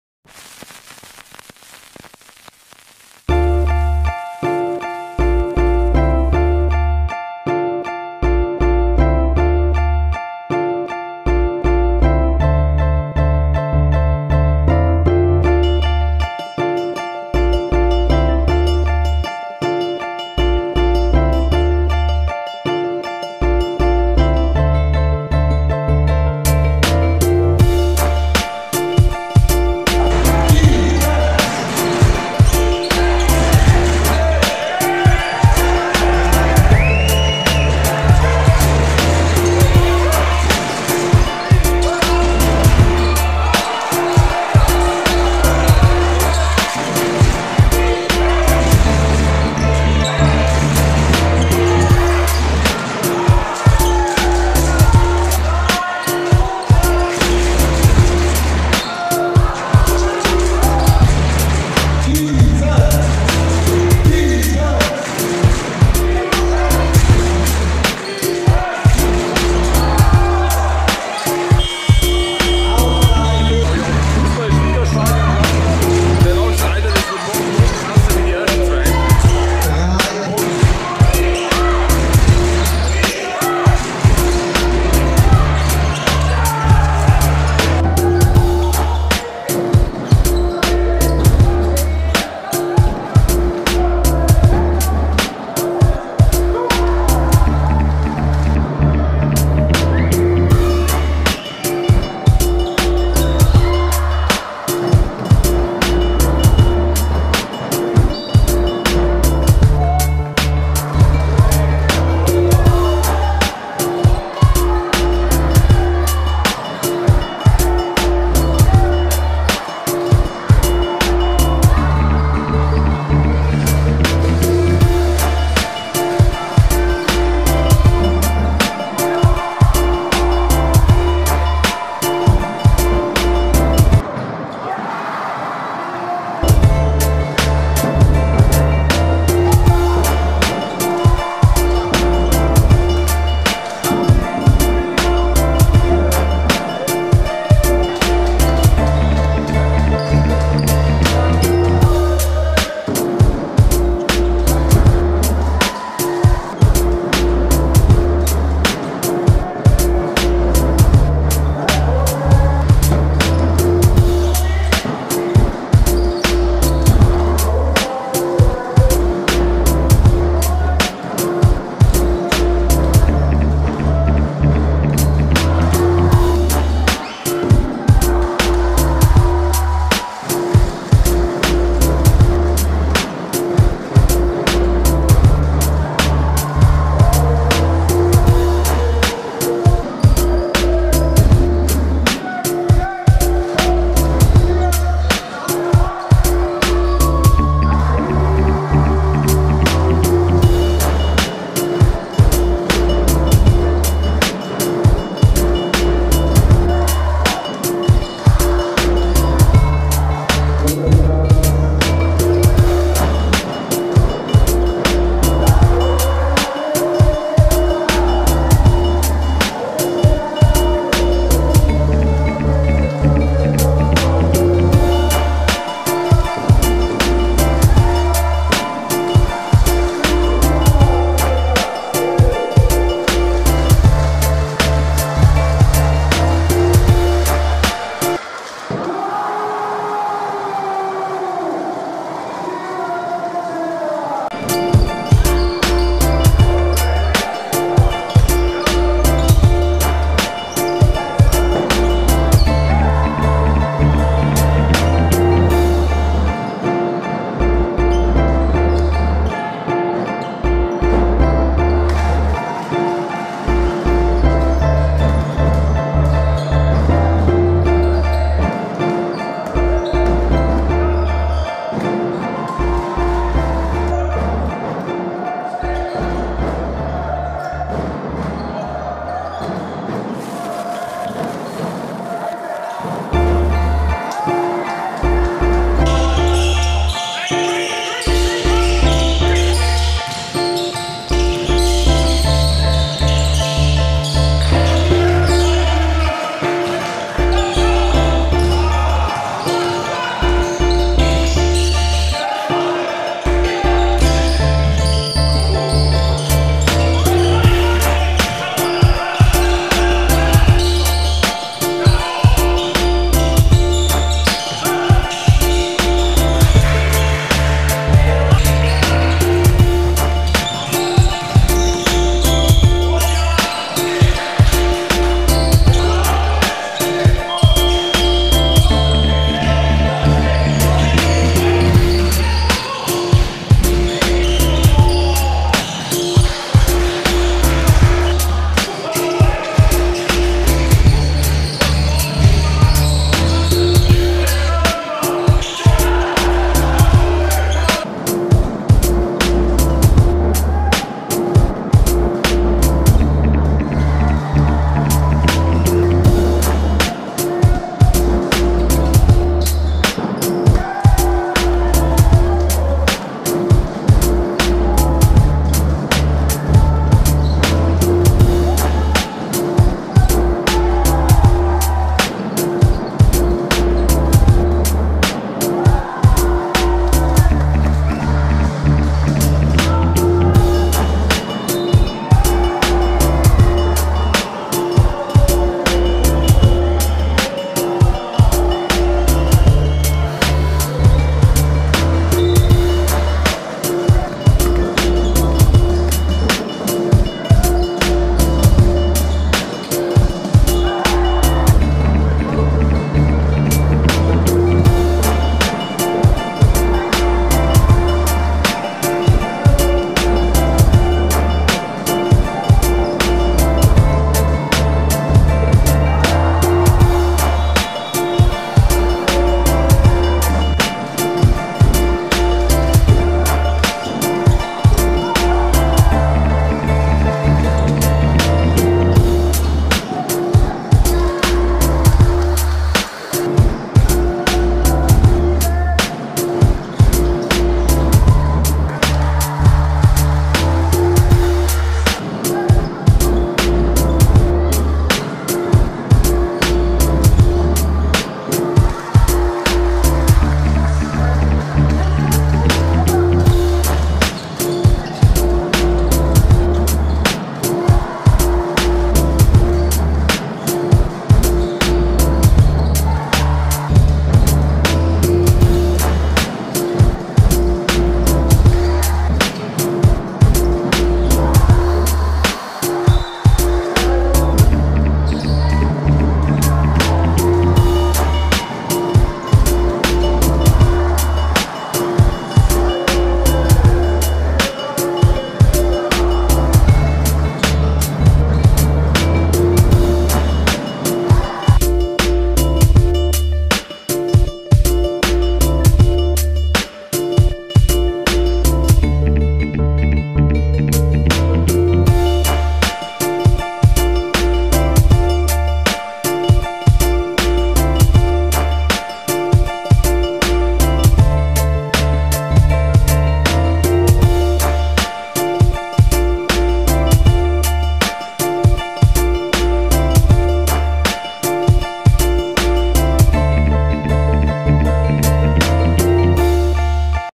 Miodrag Mirceta - Germany - Bad Aibling Fireballs - 1.regionalliga 2016/17
Miodrag Mirceta

28 years old

194.5cm

SG/SF

Bad Aibling Fireballs 2016/17
Season stats:
15.2 ppg
6.1 reb
22.3 min.per game
83.3% free throw
48.5% 2 pts
46.4% 3 pts
Last 6 games:
23.3 pts
8.9 reb
1.5 asist
57% for 3 pts!